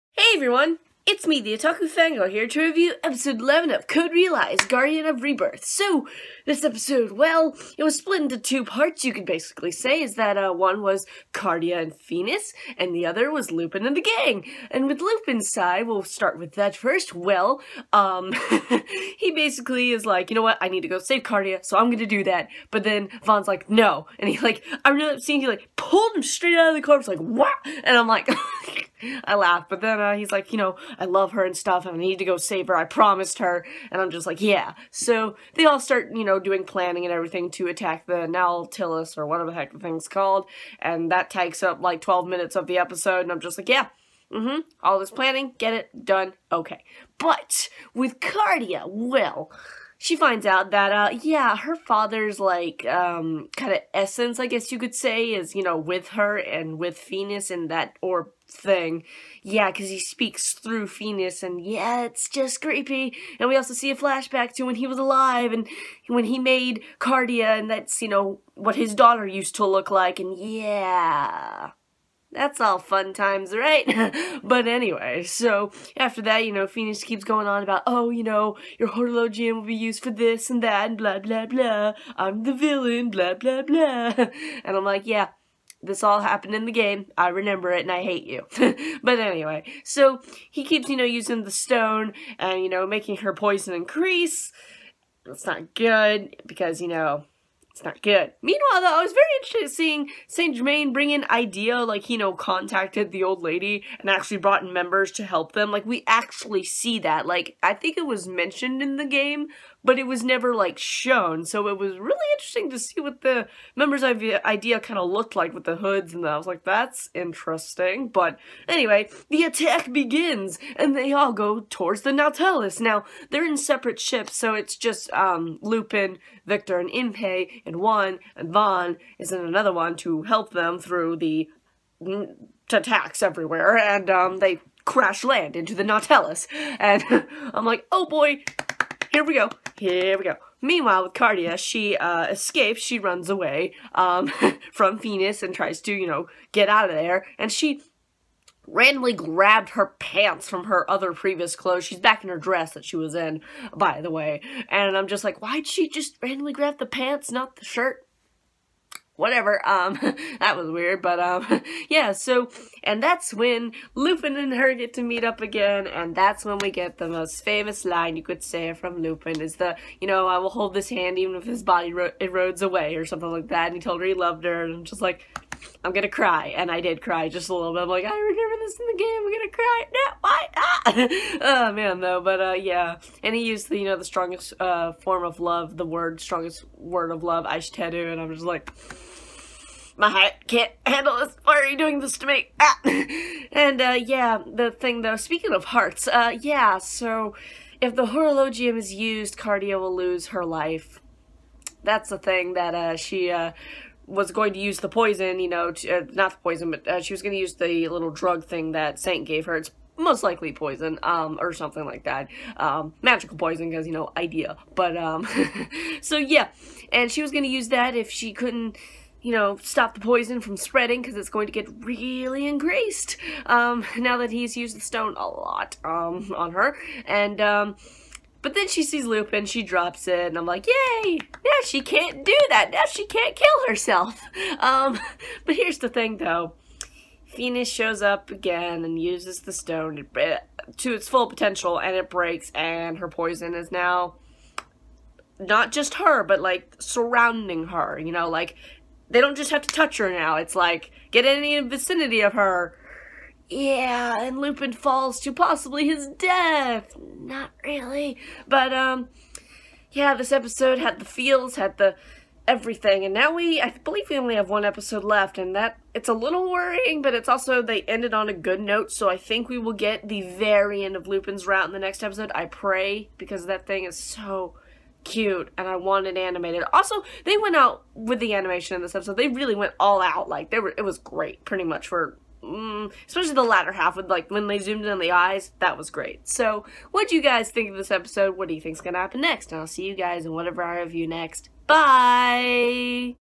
Code: Realize ~Guardian of Rebirth~ Episode 11 Review "Love Confession"
HE SAID IT! Now for the love of everything please give us that beautiful end to Lupin's route next episode..